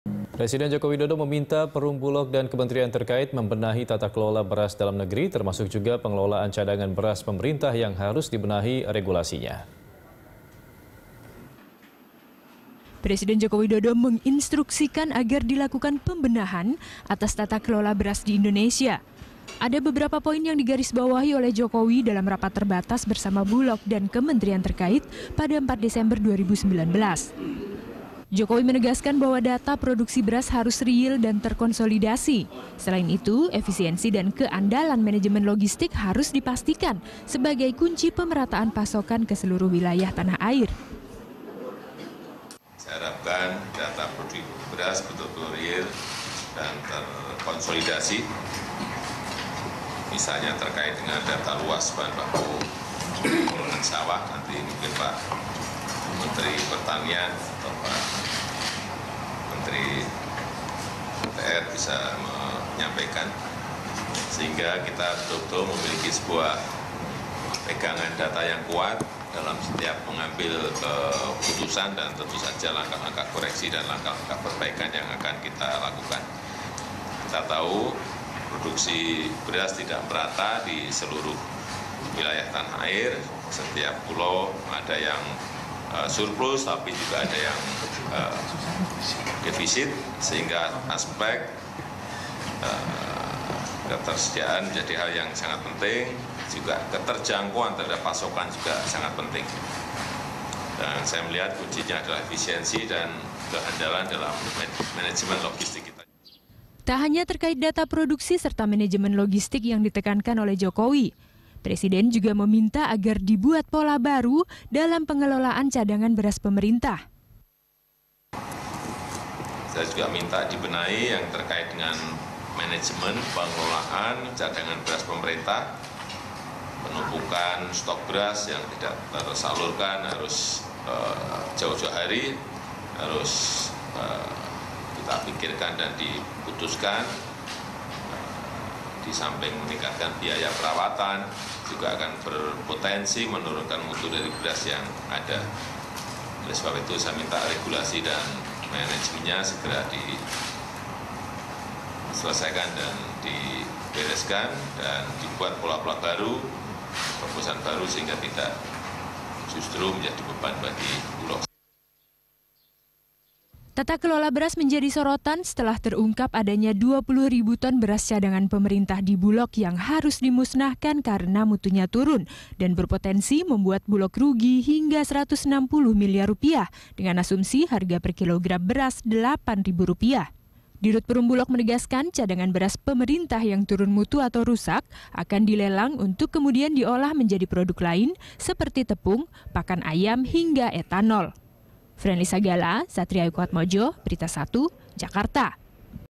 Jokowi Minta Bulog dan Kementerian Benahi Tata Kelola Beras
Presiden Joko Widodo meminta Perum Bulog dan kementerian terkait membenahi tata kelola beras dalam negeri, termasuk pengelolaan cadangan beras pemerintah yang harus dibenahi regulasinya.

Facebook.com/BeritaSatu
Youtube.com/BeritaSatu